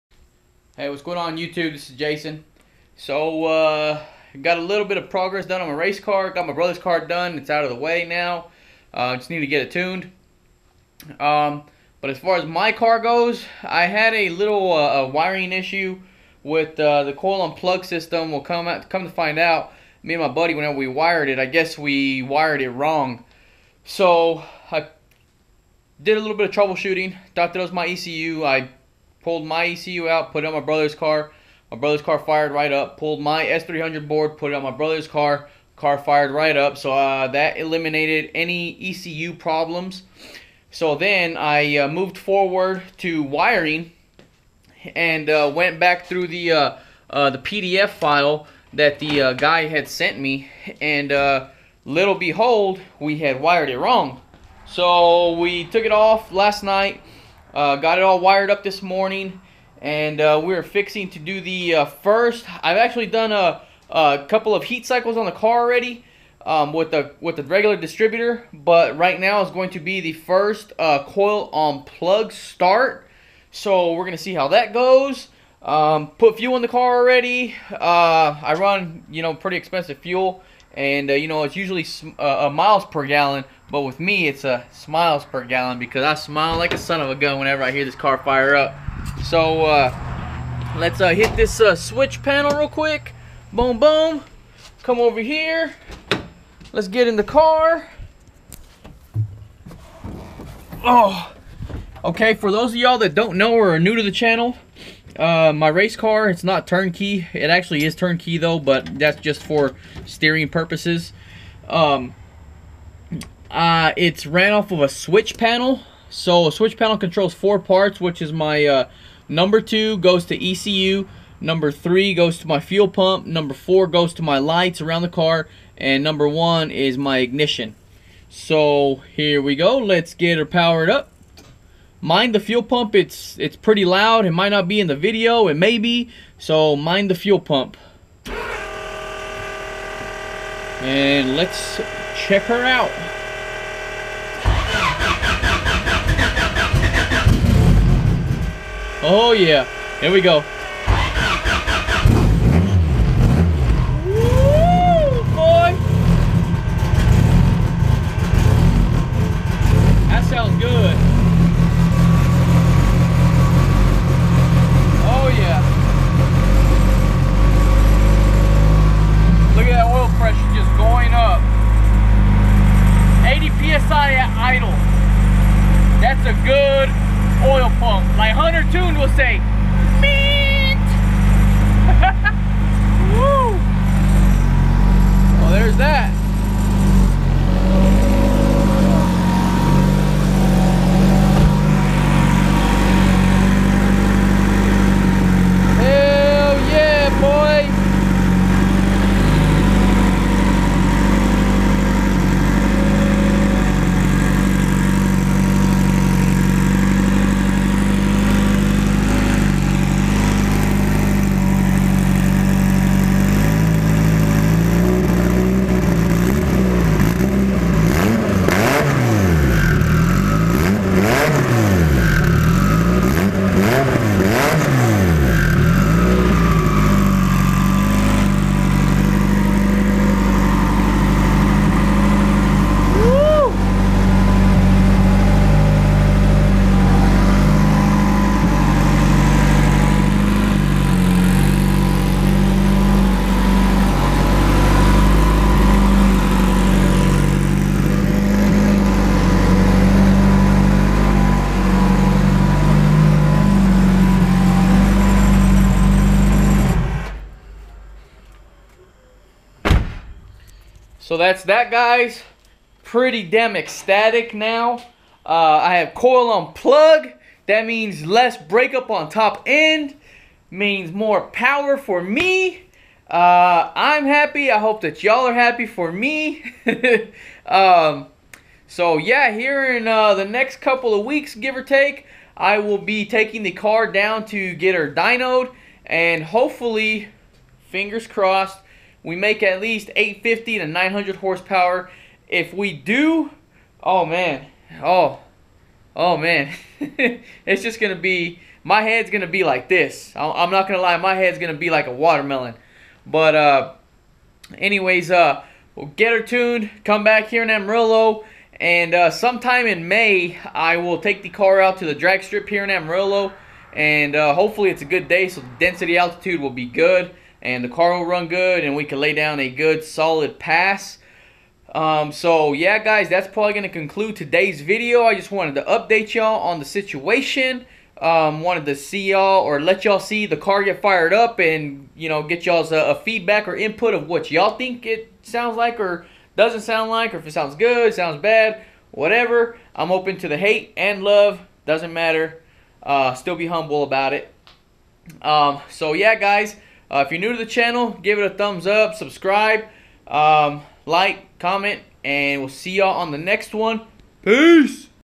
First start up of my coil on plug setup, skunk2 pro1 cams revving up!!
Gsr turbo race car